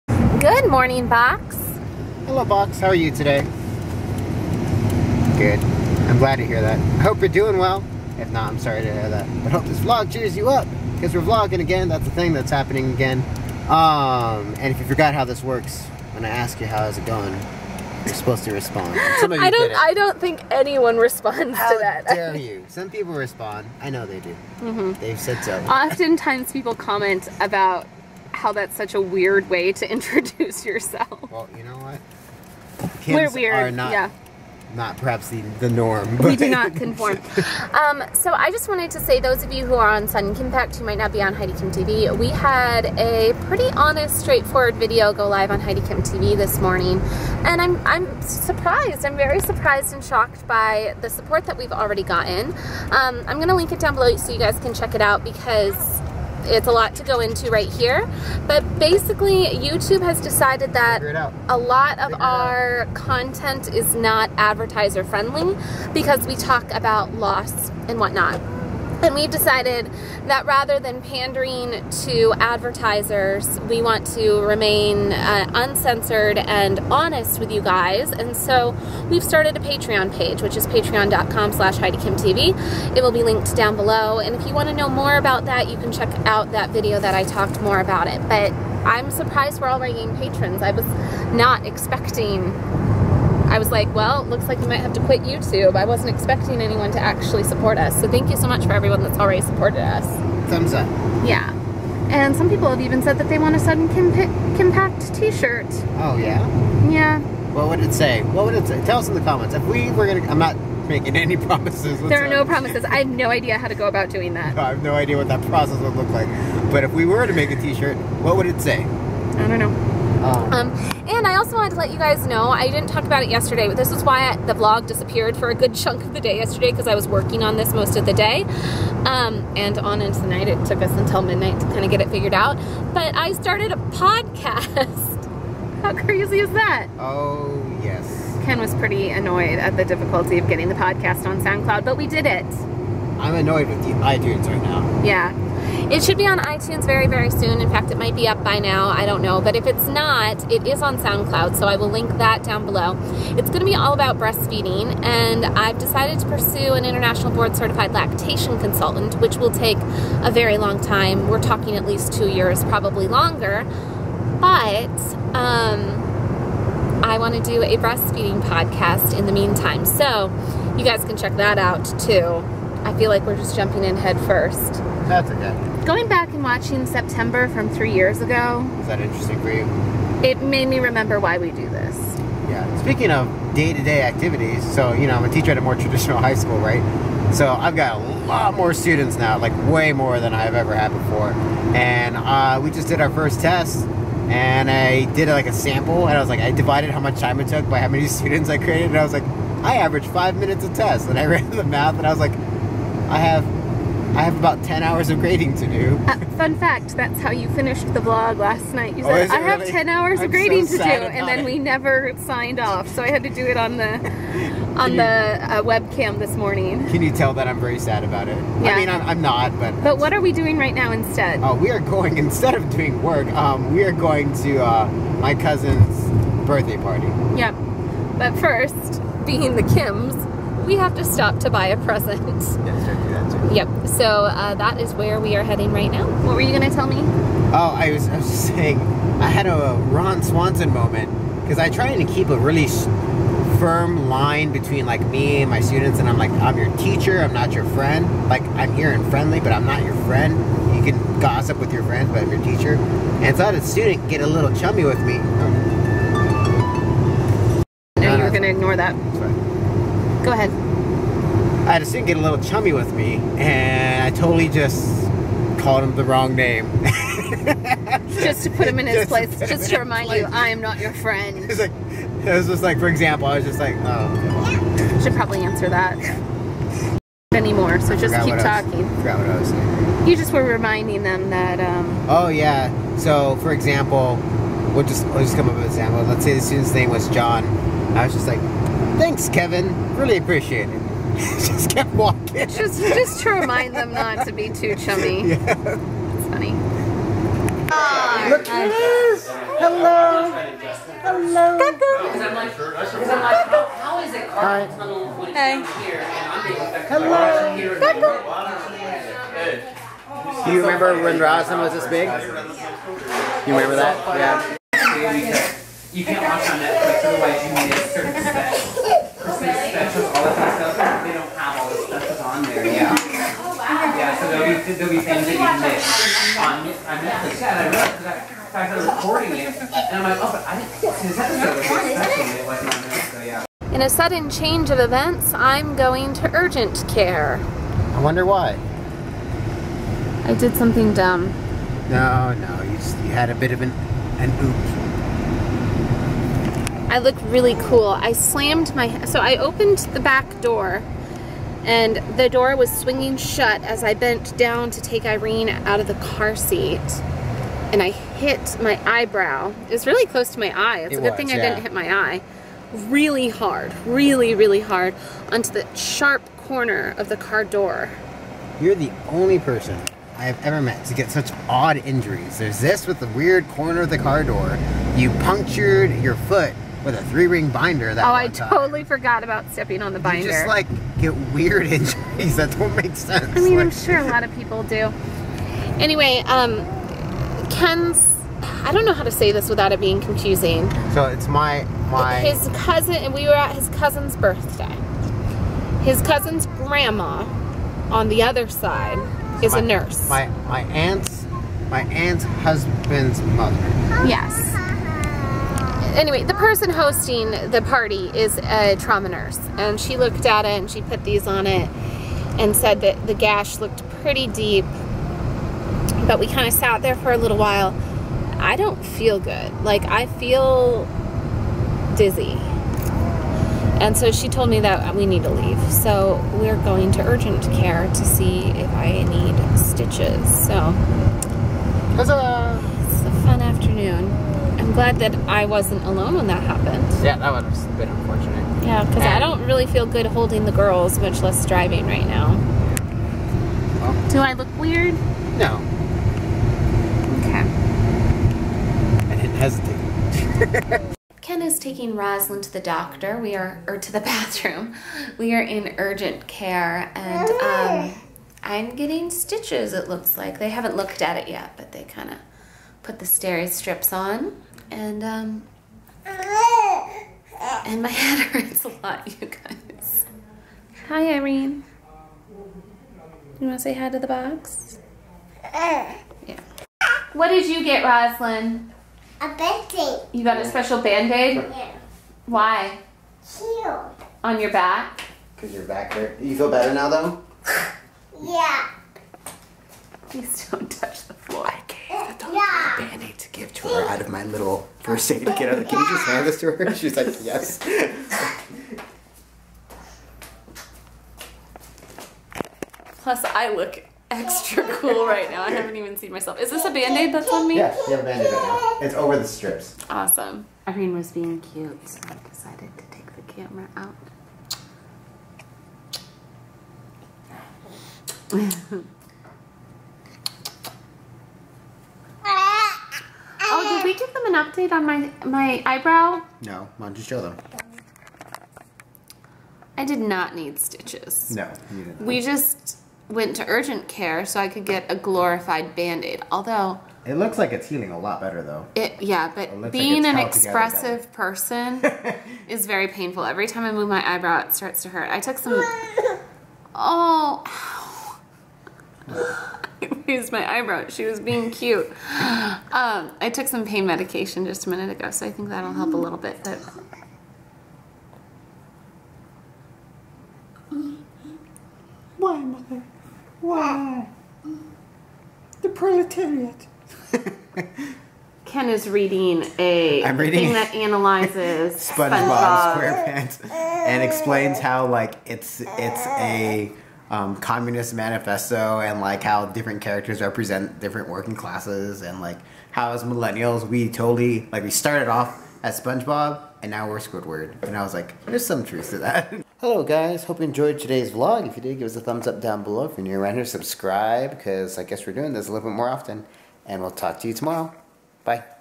A TRIP TO URGENT CARE
Heidi injures herself and goes to Urgent Care,  little cousins birthday, and how to make us sustainable.